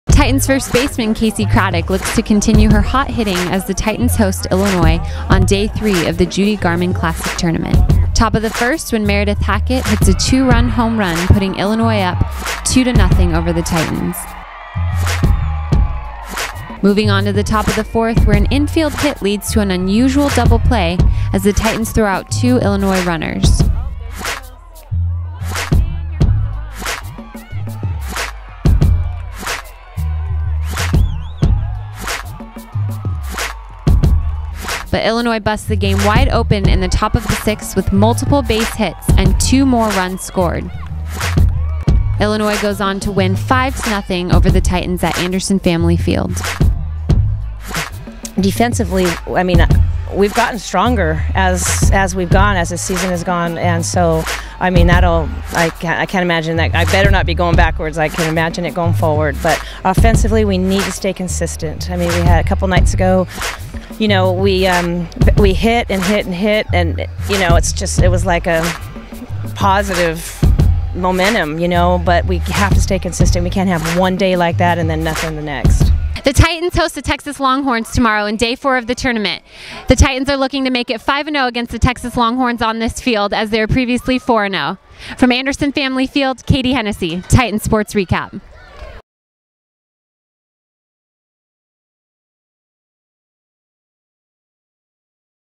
Softball: Judi Garman Classic Day 3
By Katie Hennessey, Althea Wagoner & Derek Opina: The Titans take on Illinois after going 1-1 in their first two games of the Judi Garman Classic